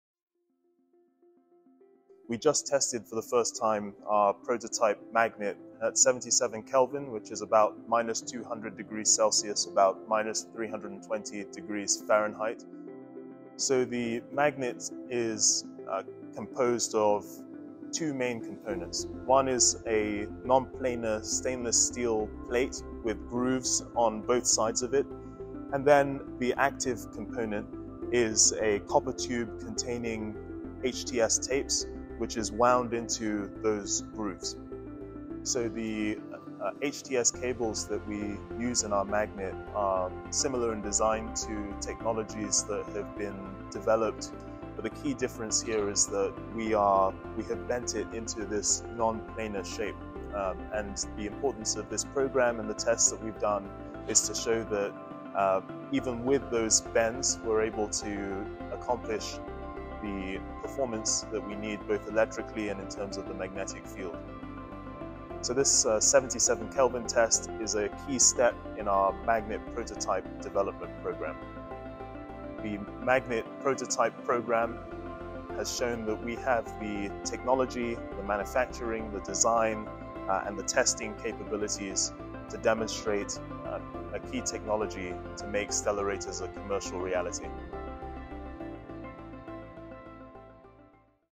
How Type One Energy Is Revolutionizing Stellarator Magnets
Eager to dive deeper into our magnet technology? Hear from HTS R&D Engineer Richard Ibekwe as he explains the significance behind our 77K test and groundbreaking non-planar magnet technology.